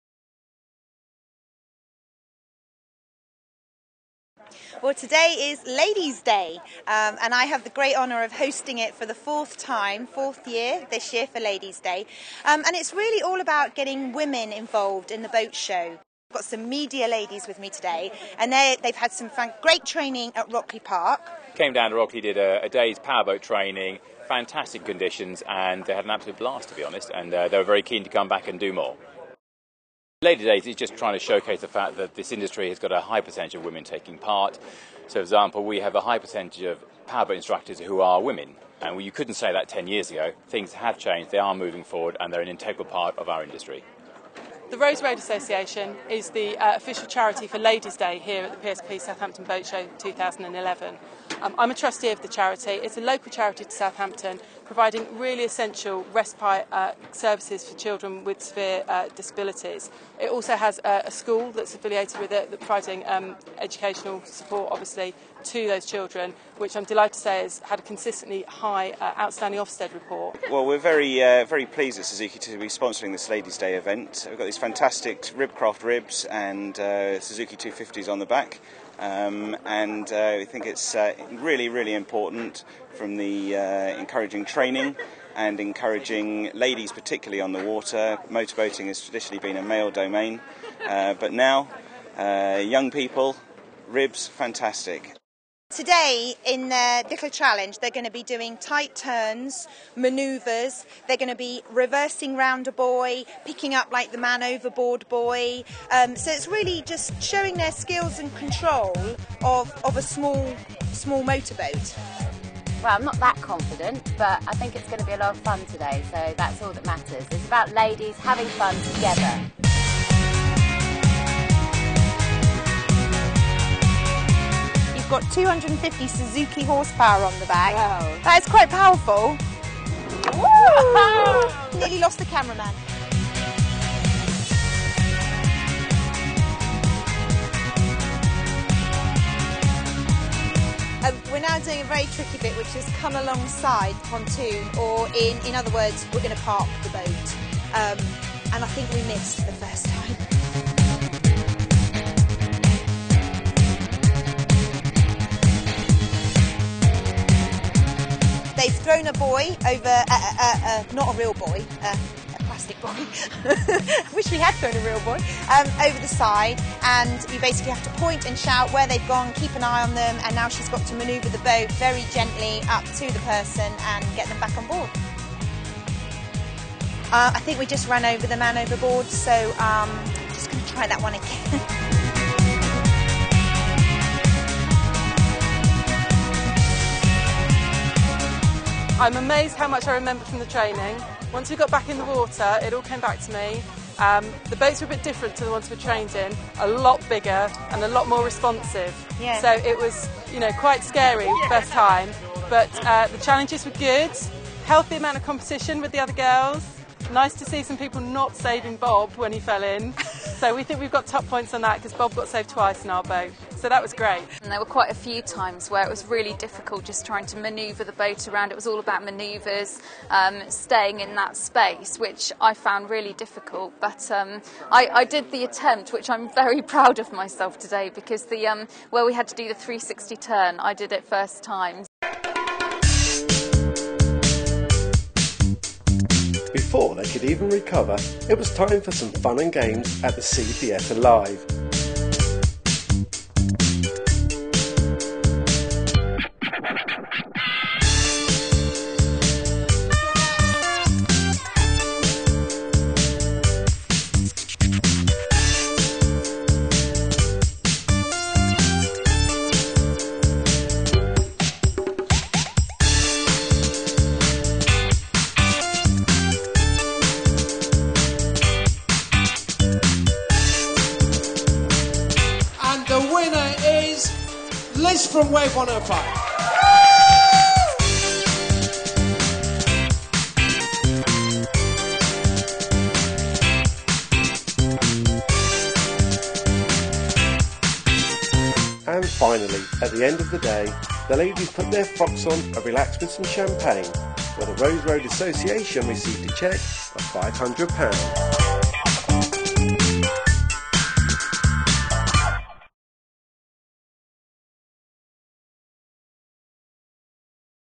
Ladies Day
See a review of Ladies Day at the PSP Southampton Boat Show